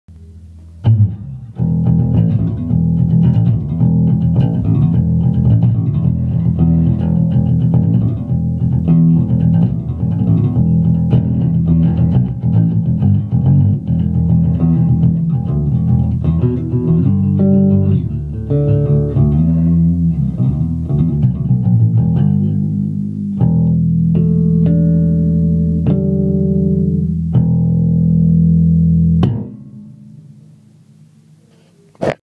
J'Z STUDIO千歳烏山店1.2.3stのEBS Classic450+ClassicLine 810
J'Z STUDIO千歳烏山店1.2.3stに常設のEBS Classic450+ClassicLine 810のサウンドです！
スタジオ選択の参考にして下さい。